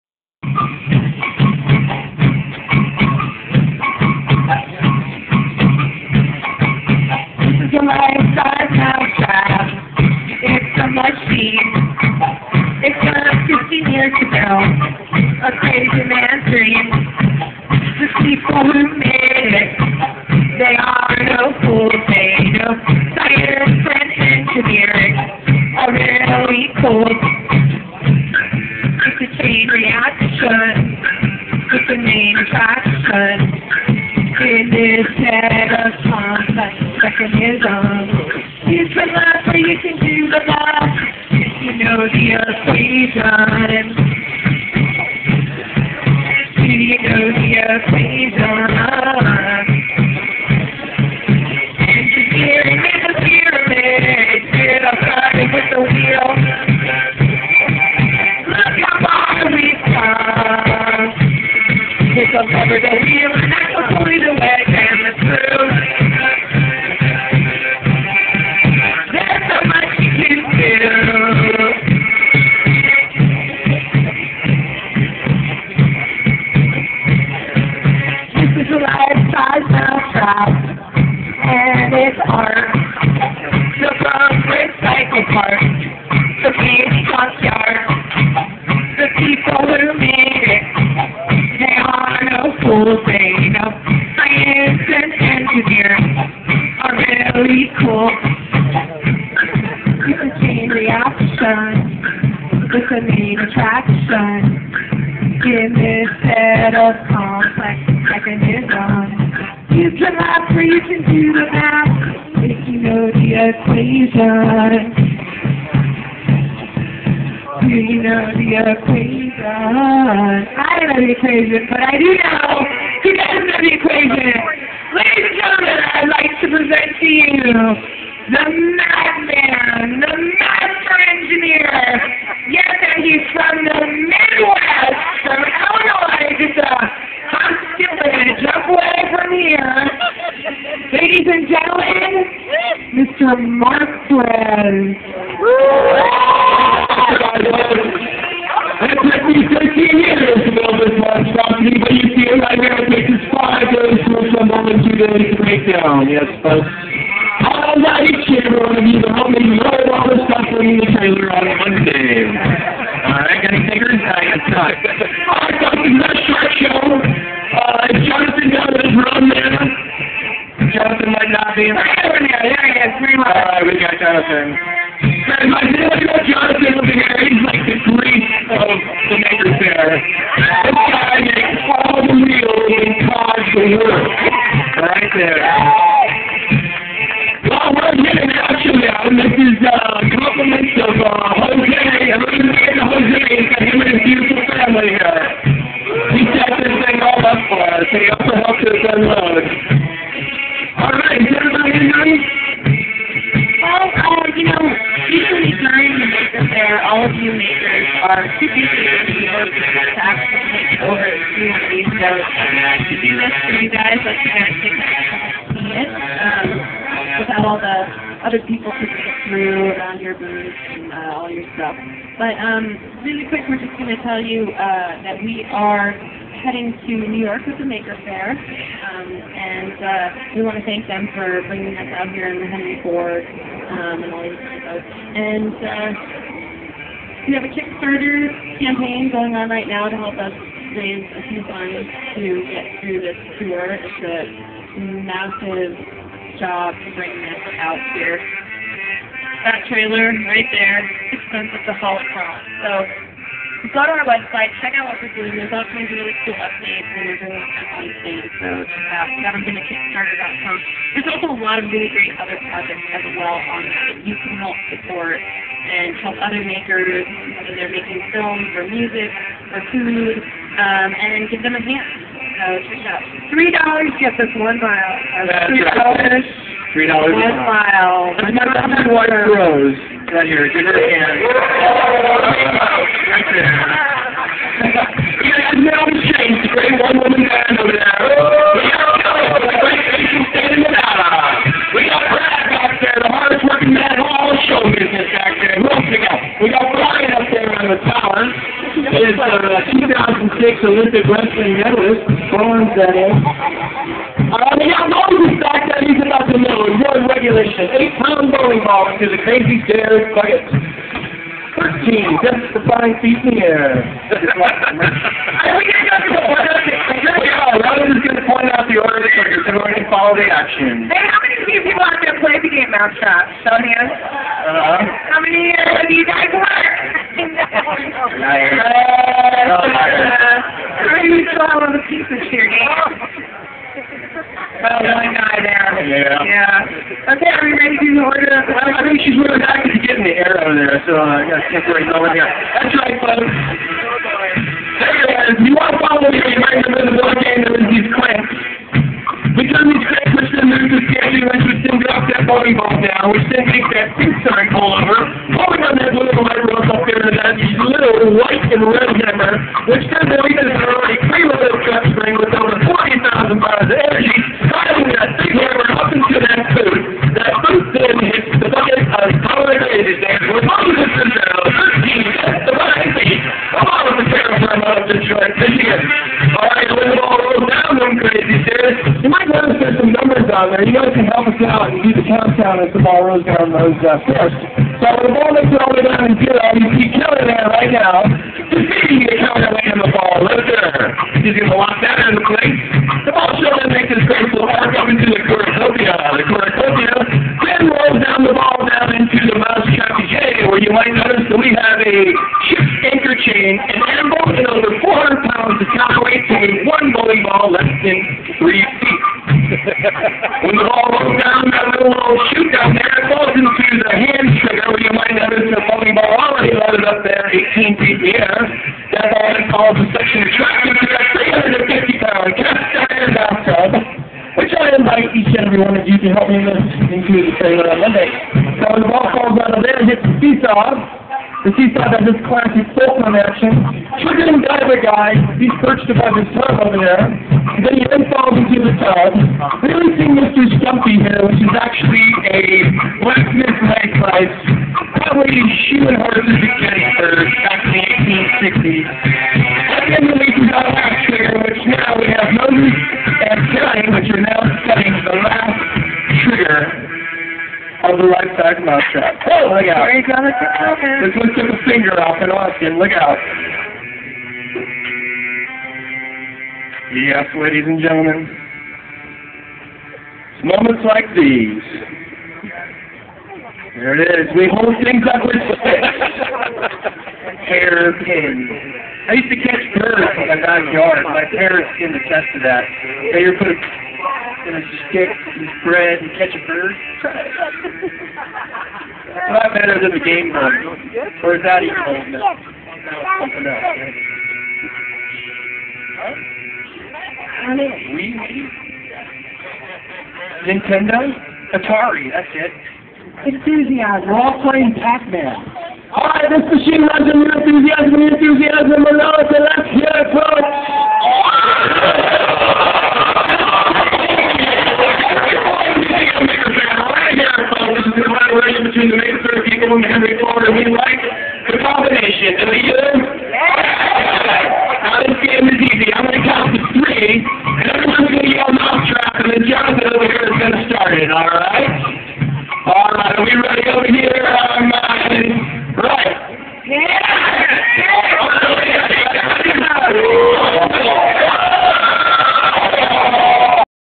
Detroit Maker Faire 2010 - Giant Mouse Trap Introduction
The introduction to the giant Mouse Trap game demonstration at the 2010 Detroit Maker Faire in Dearborn, MI

Forgive me for the worst sound ever...this was recorded on my phone, so quality isn't exactly high on the list of things that describe this video.